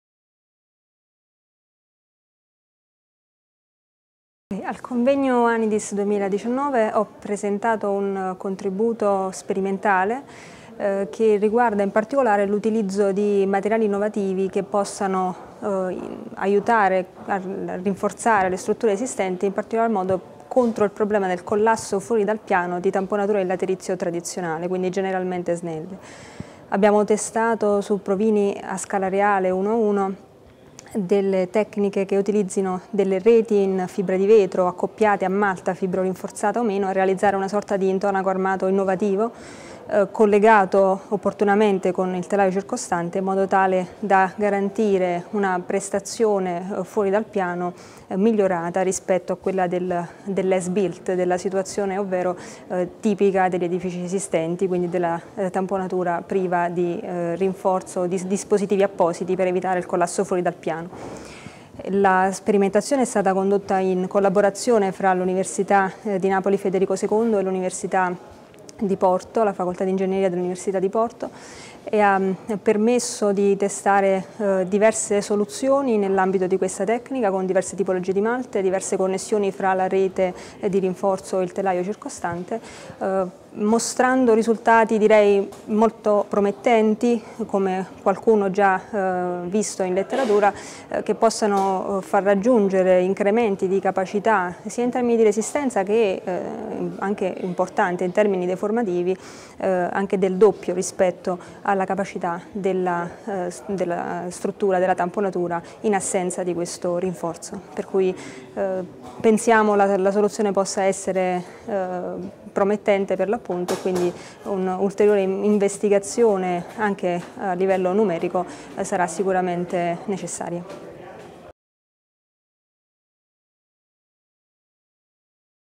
XVIII Convegno ANIDIS: Maria Teresa De Risi
Experimental analysis of Textile Reinforced Mortars strengthening strategies against the out-of-plane collapse of masonry infill walls

Maria Teresa De Risi, André Furtado, Hugo Rodrigues, José Melo, Gerardo Mario Verderame, António Arêde, Humberto Varum, Gaetano Manfredi